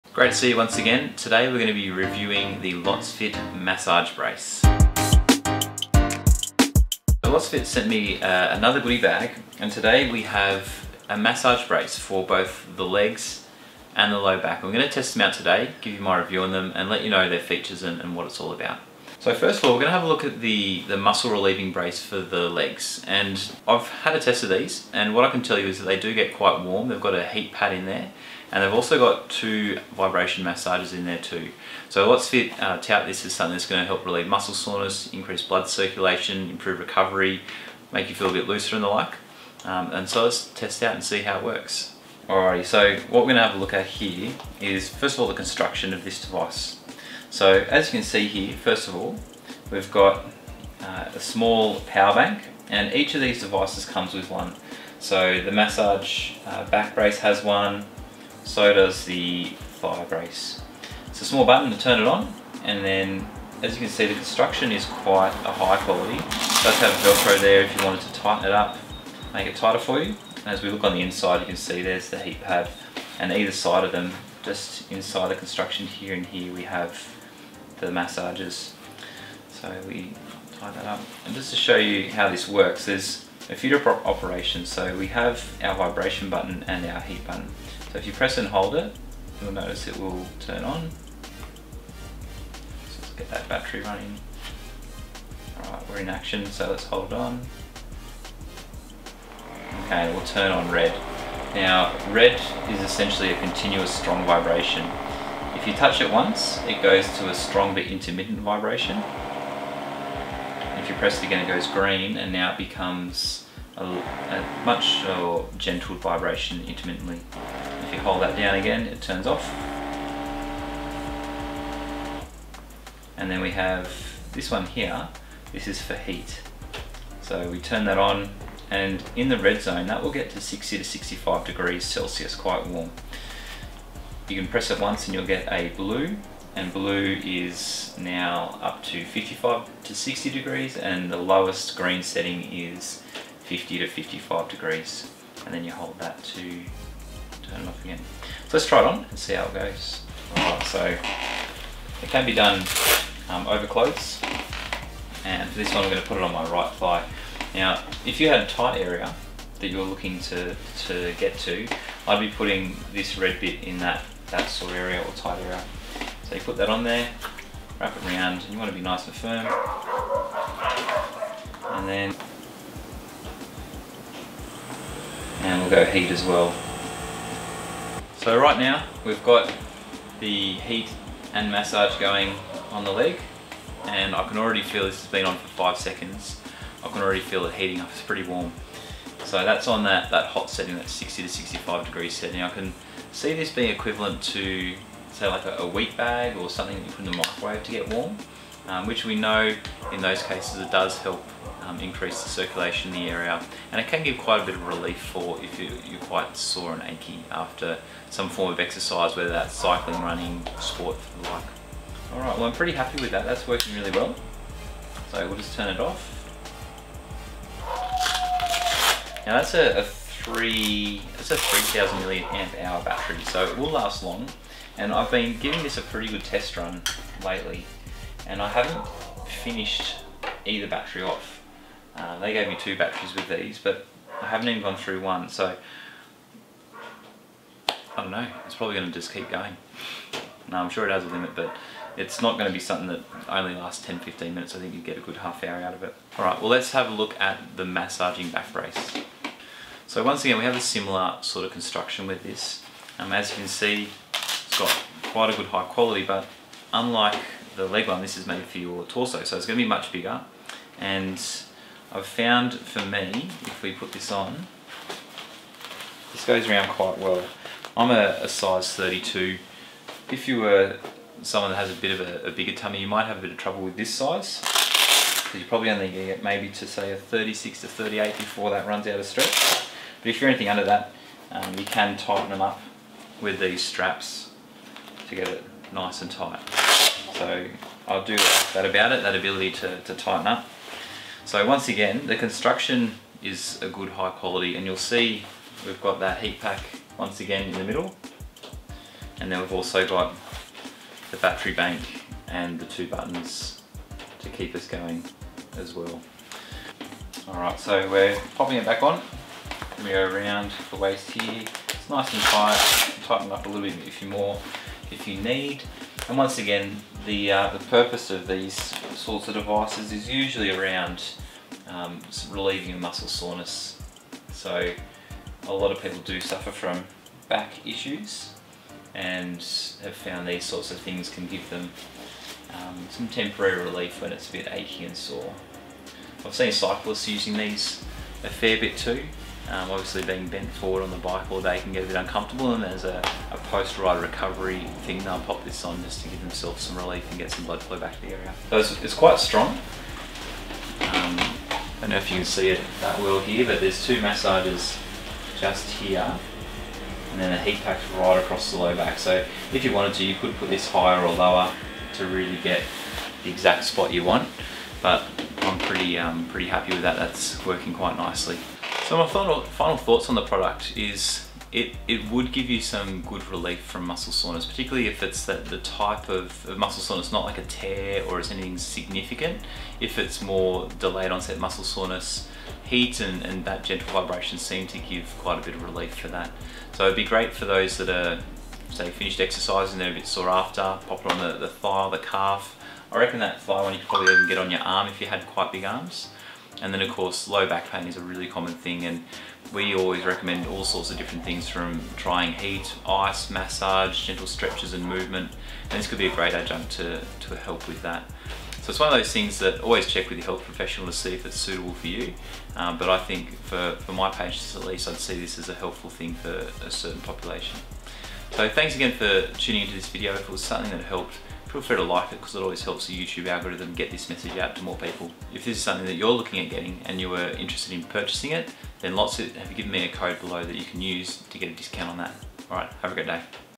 HEAT & MASSAGE BACK BRACE | Test and review the Lotsfit heating belt | lower back and thigh muscles
In this video we take a look at LotsFit's heating belt. We tested both the belt for the lower back and lumbar spine as well as the smaller version for the leg and thigh muscles.